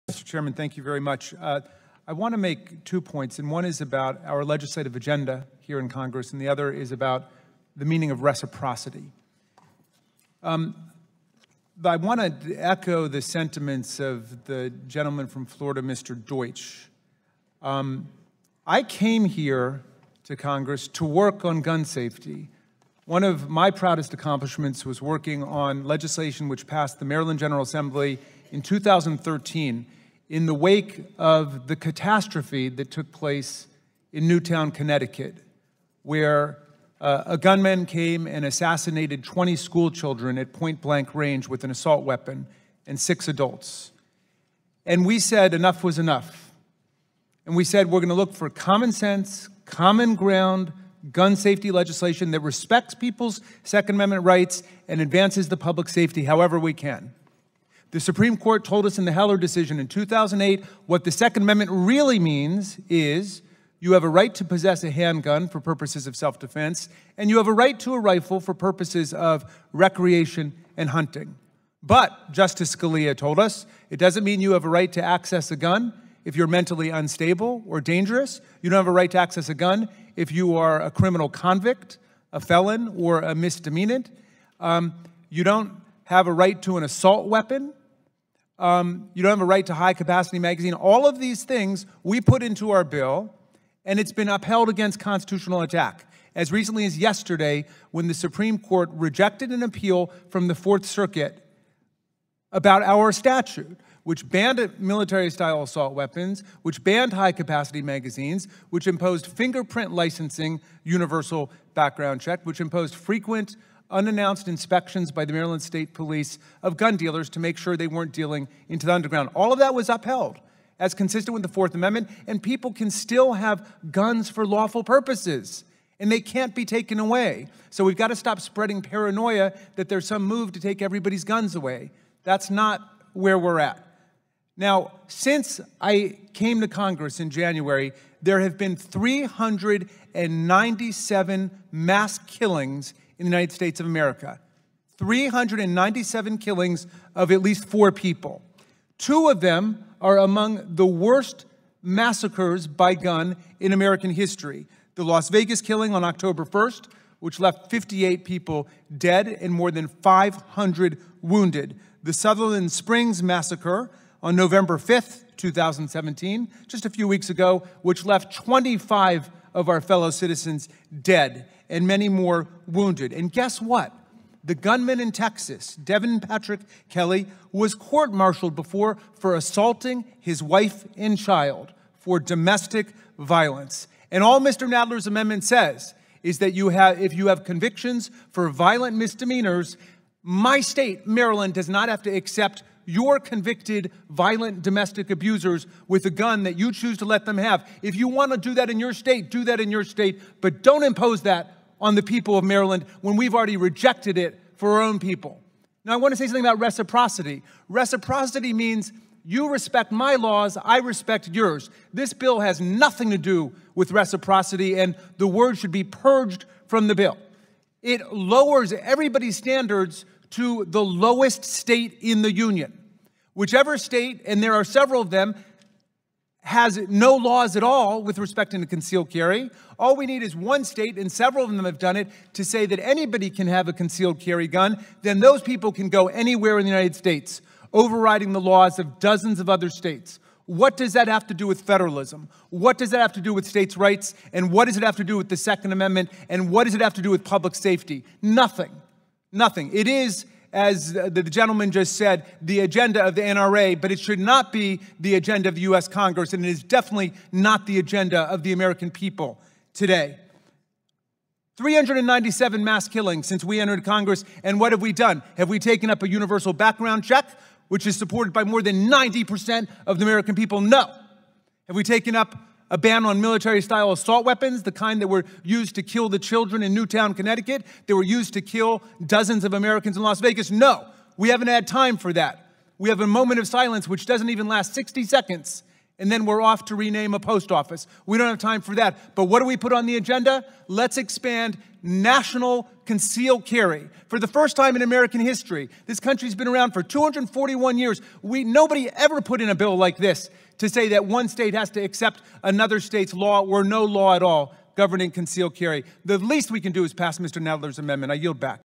Congress' Legislative Agenda on Guns
On Sept. 29, 2017, House Judiciary Committee took up a shockingly dangerous bill to nationalize concealed carry. The dreadful concealed carry bill, H.R. 38, is an assault on federalism that would override gun safety laws in nearly every state, forcing every state to give gun rights to people who get them in states with the weakest standards and most lax practices. A federal concealed carry law is an outrageous encroachment on states' rights, especially states with strong gun safety laws like Maryland.